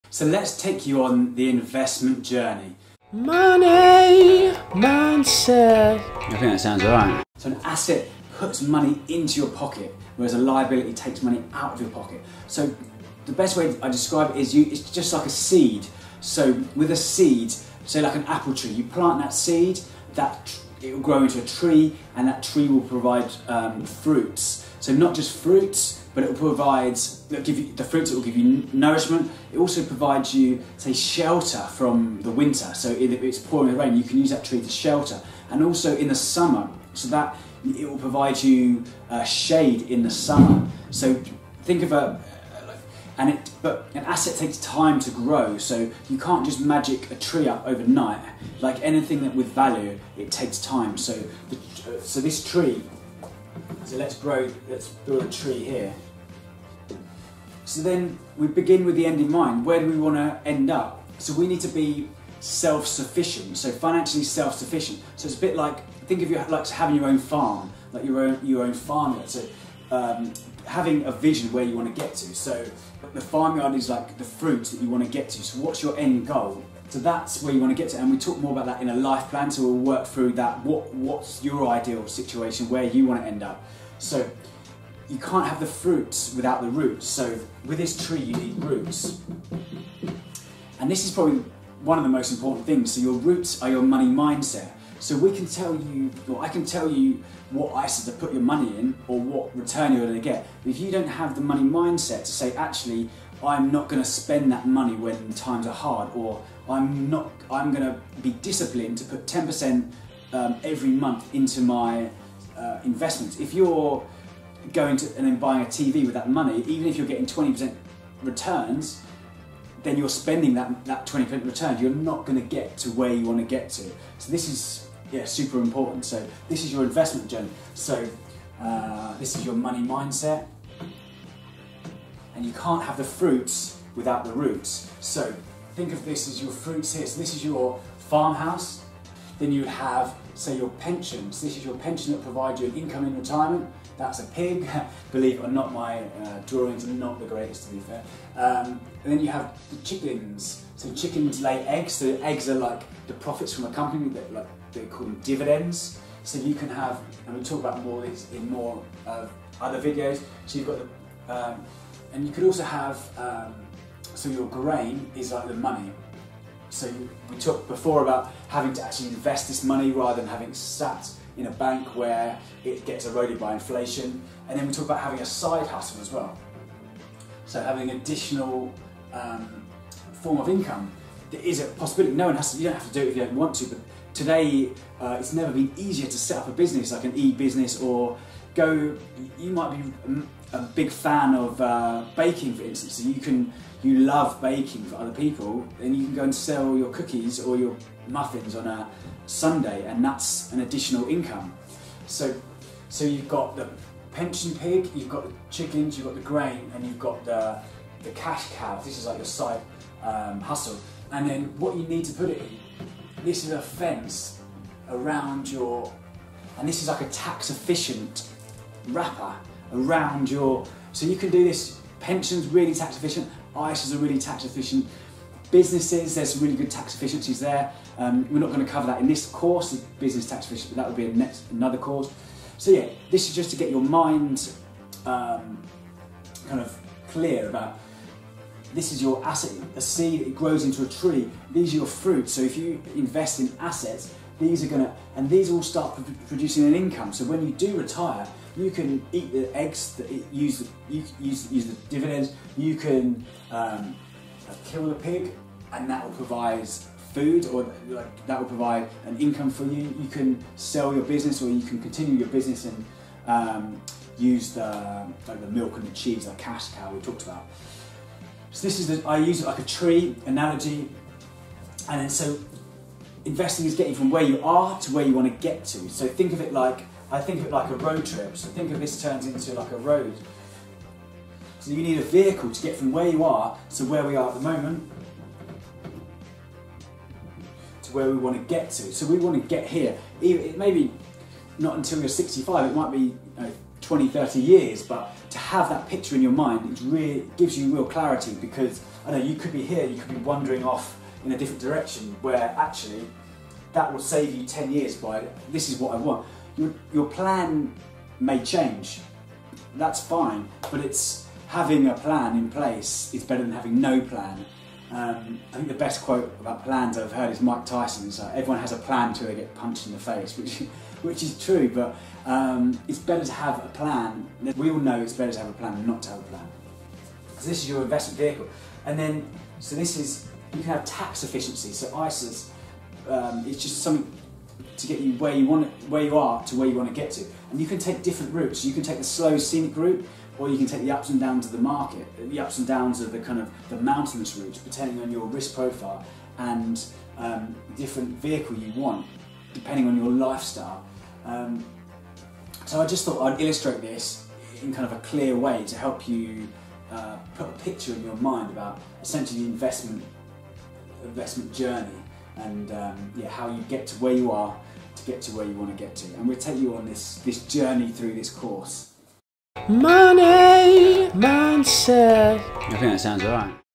Investing for Beginners. How to Invest. What is the Investment Journey?
Investing for beginners can feel confusing, but when you know which investments to focus on, the investing journey becomes easier.

Are you ready to take control of your financial future and learn how to invest?

Learn how to invest and discover what an asset is verses what is a liability

Watch this video on investing for beginners today to find out